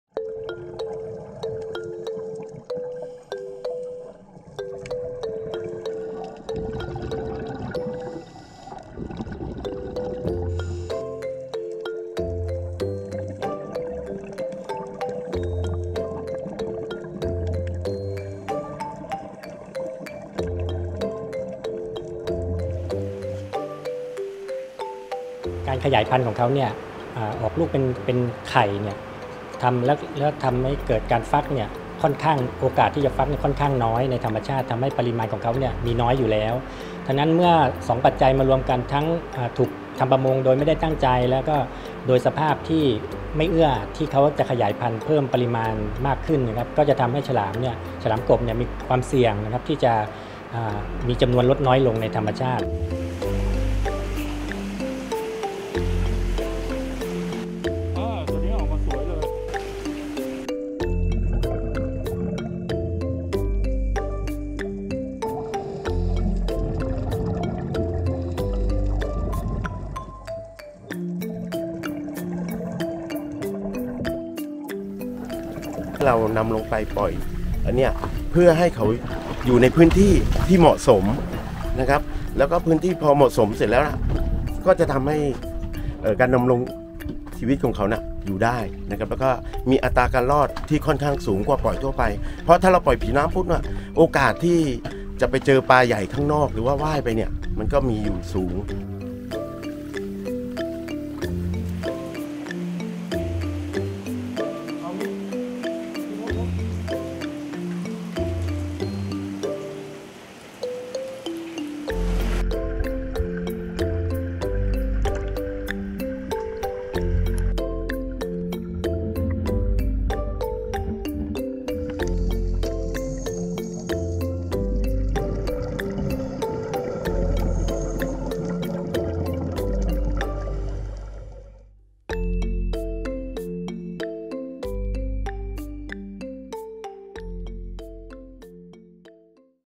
Thailand breeds baby bamboo sharks to save the species from extinction
Scientists in Thailand have been breeding and releasing baby bamboo sharks to help the endangered species survive. They are one of the smallest and least dangerous species of sharks native to the Gulf of Thailand, but bamboo sharks have become endangered because of their popularity among collectors and exotic diners. Researchers hope to increase the population of bamboo sharks and remove the species from the International Union for Conservation of Nature’s “Near Threatened” list.